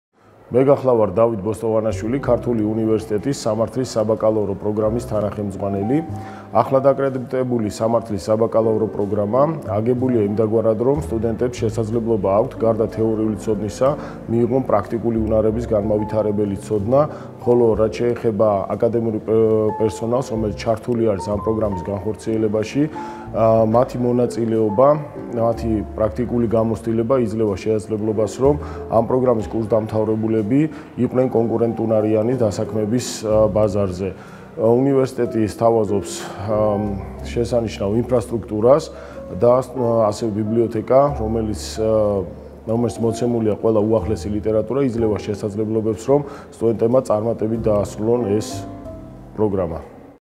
პროფესორი დავით ბოსტოღანაშვილი საბაკალავრო პროგრამა "სამართლის" შესახებ
პროგრამის კოდი: 1450103

სამართლის საბაკალავრო პროგრამა შესანიშნავი შესაძლებლობაა მათთვის, ვისაც სურვილი აქვს, ჩამოყალიბდეს მაღალკვალიფიციურ იურისტად და ჰქონდეს წარმატებული კარიერა. პროგრამა შემუშავდა სახელმწიფოს მიერ დადგენილ სამართლის დარგობრივ მახასიათებელზე დაყრდნობით, რომელიც ზუსტად განსაზღვრავს, კონკრეტულად რა უნდა იცოდეს, რისი გაკეთება უნდა შეეძლოს და რა ღირებულებების მატარებელი უნდა იყოს იურისტი, გაანალიზდა საუკეთესო პრაქტიკა.